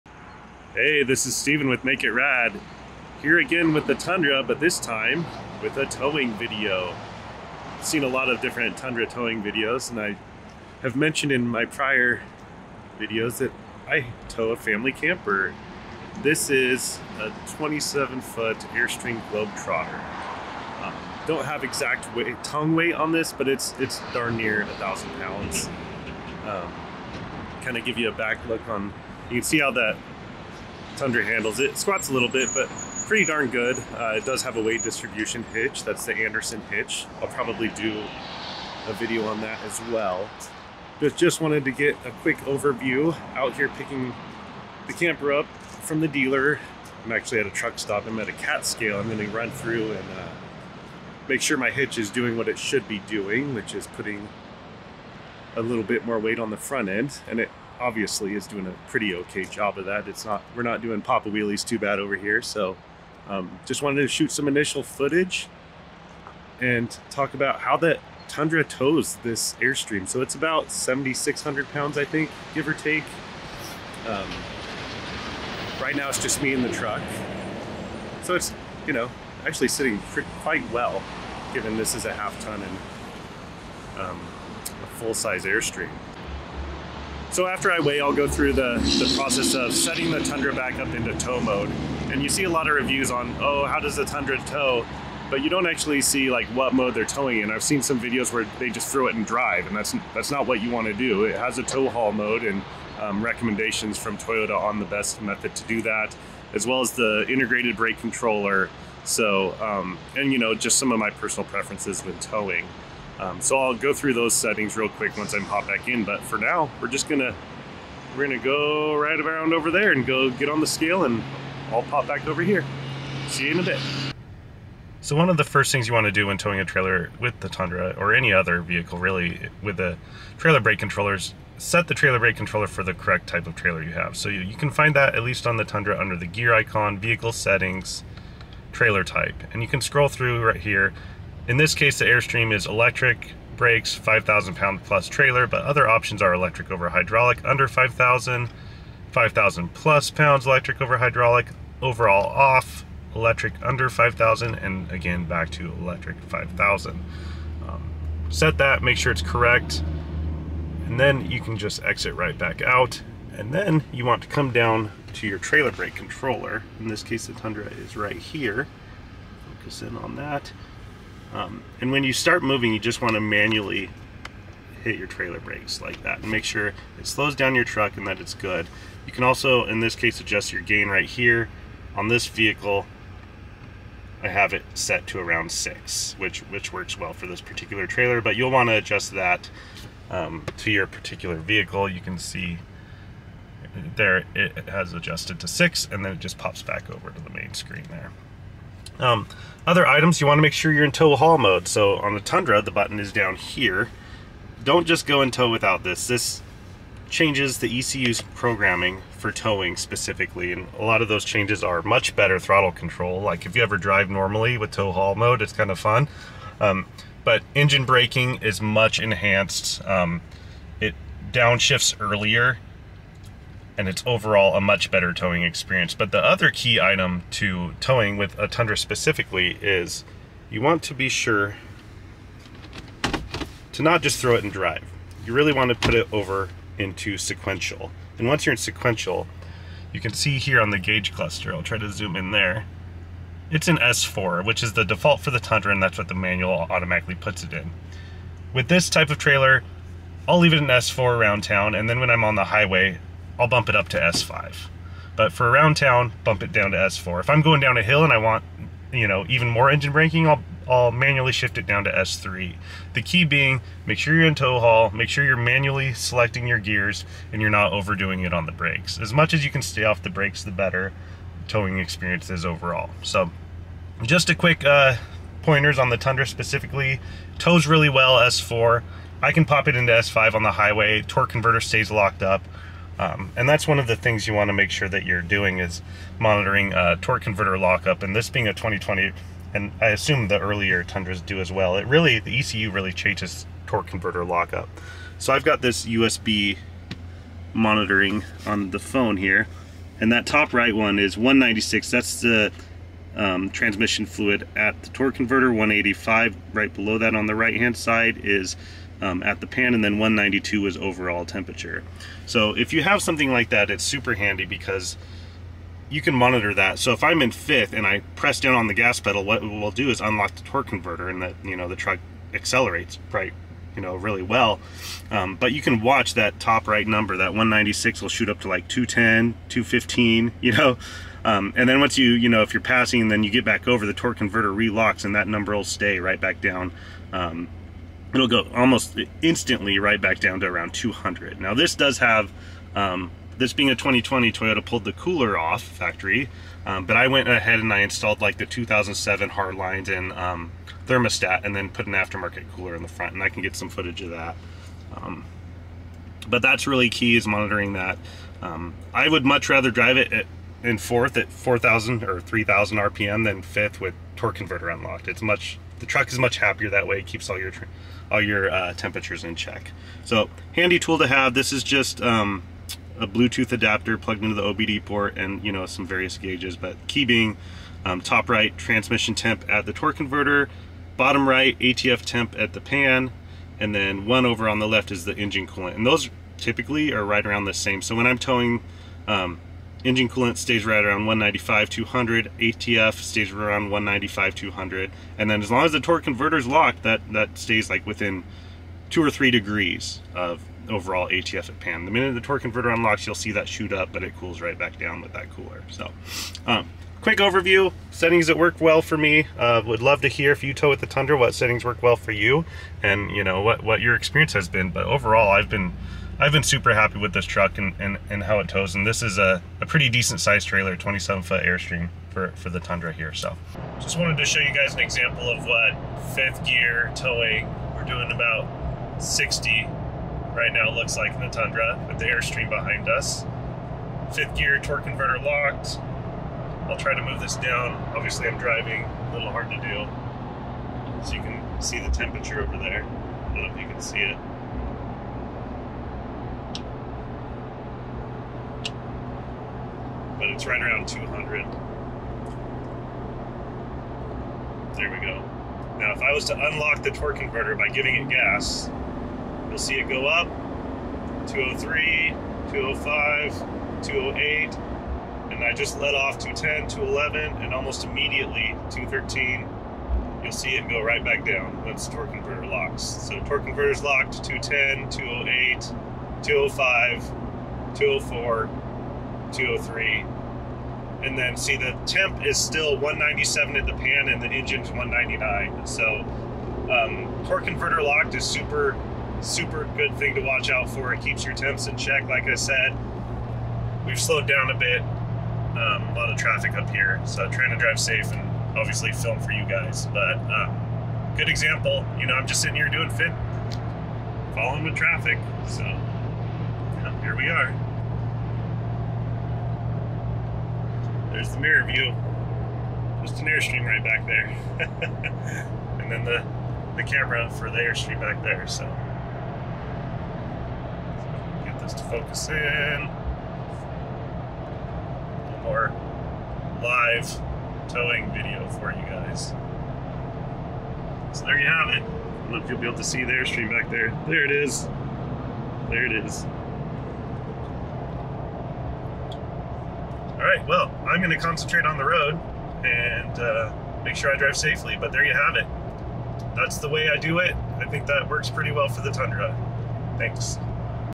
Tundra Towing Performance! How does the v8 Tundra tow a 27ft Airstream?
Overview of the Tundra performance, setup, and personal preferences for the best experience towing a travel trailer with the 2.5 gen Tundra.